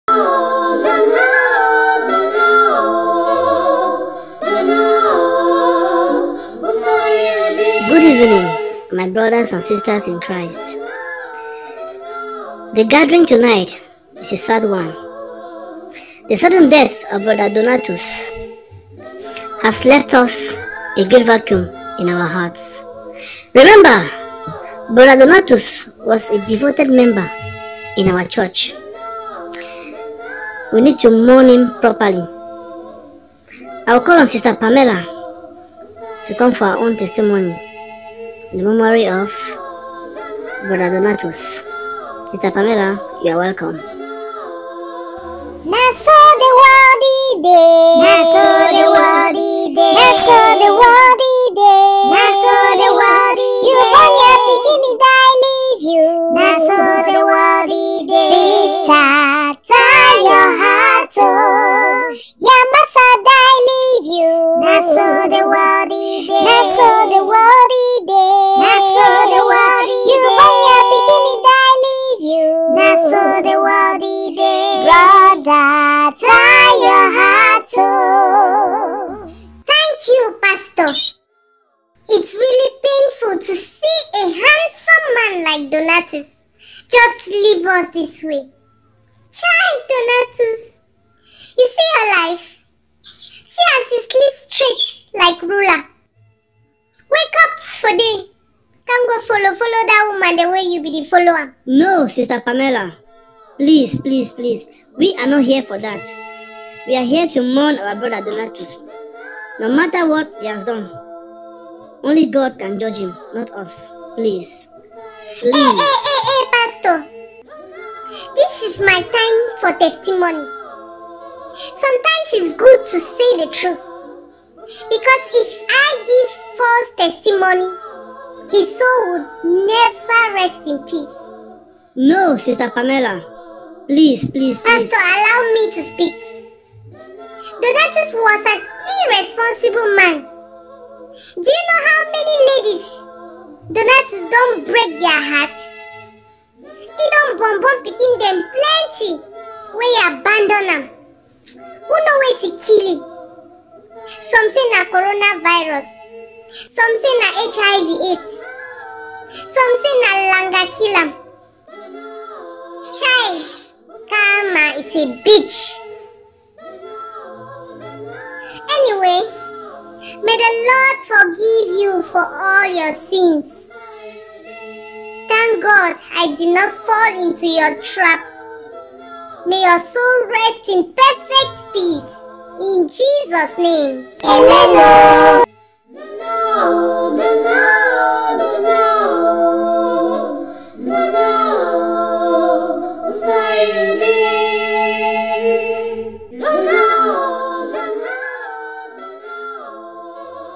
Ep. 12: THE FUNERAL "Les funérailles"
Sister Pamela's tribute to brother Donatus who passed on a few days ago.